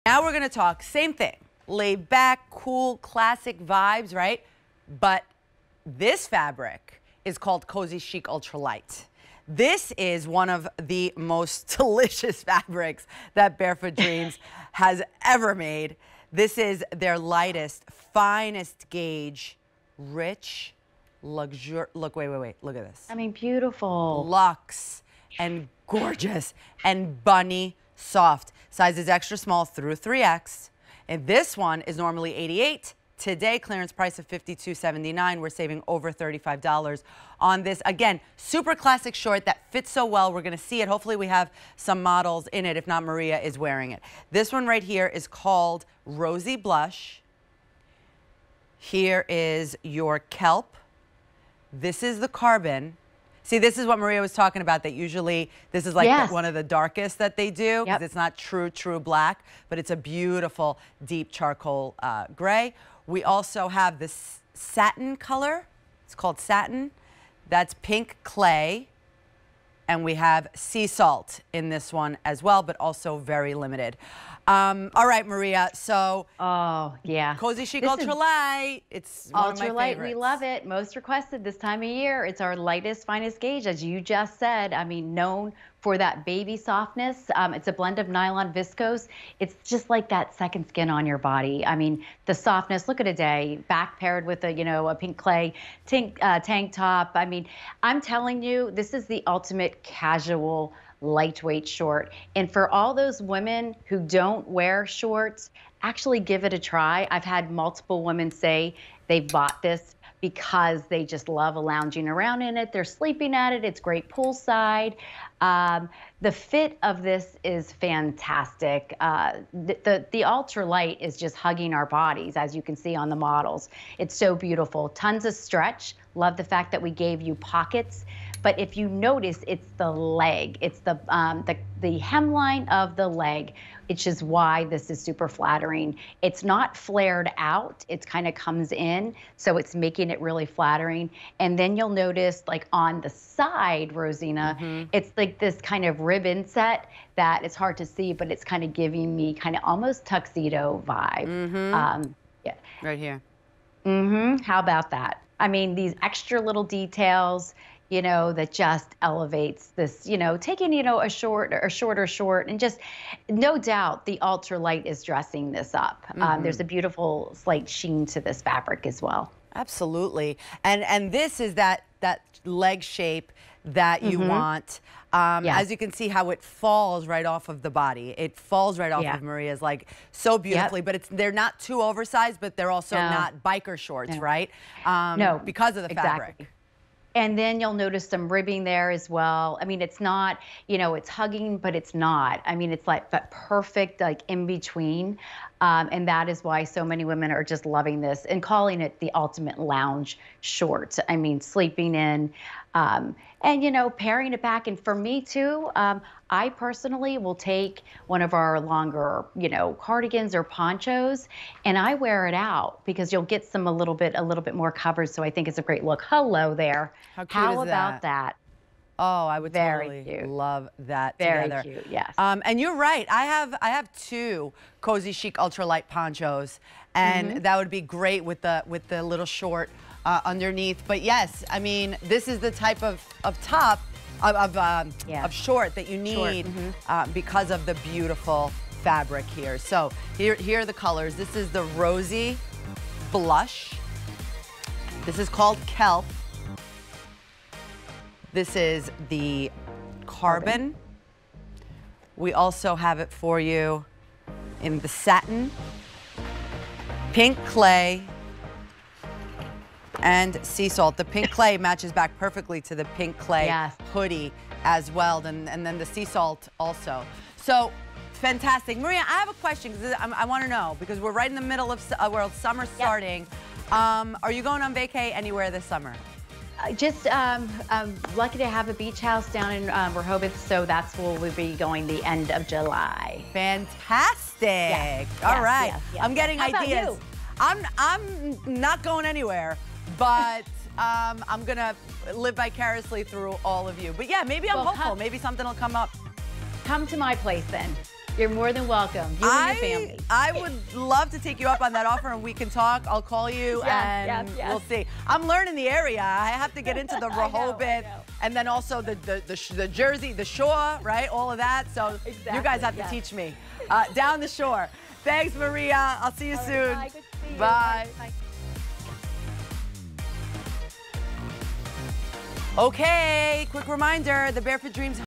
Barefoot Dreams CozyChic Ultra Lite Ribbed Inset Shorts on QVC
Barefoot Dreams CozyChic Ultra Lite Ribbed Inset Shorts

Anybody care for a cuddle sesh? These CozyChic shorts are soft, snuggly, and so ready to get comfy. From Barefoot Dreams.